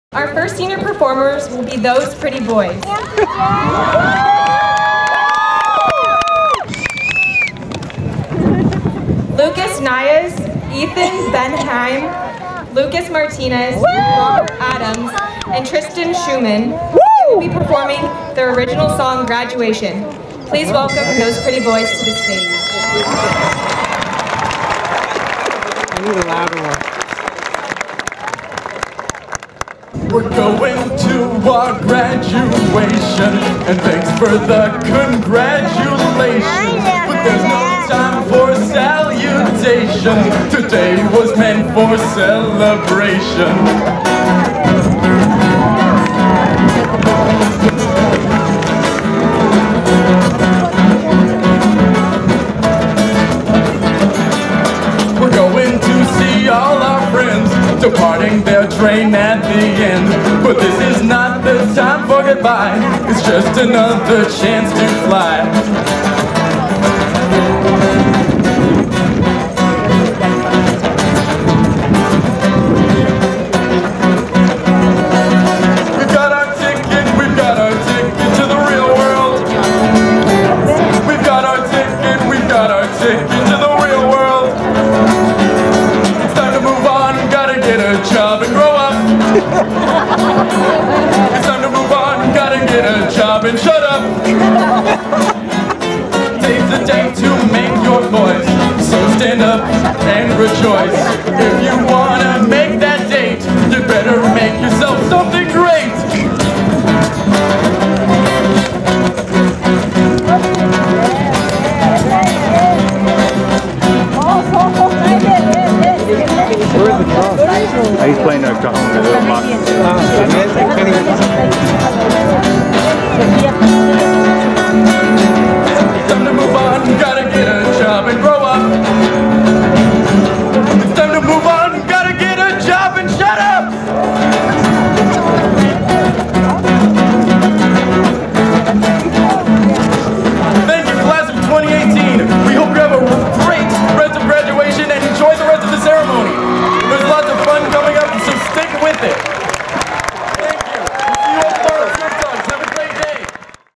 Boys Night Live! At Graduation
Boys Night performing an original song at the Huntington Beach High School 2018 Commencement Ceremony!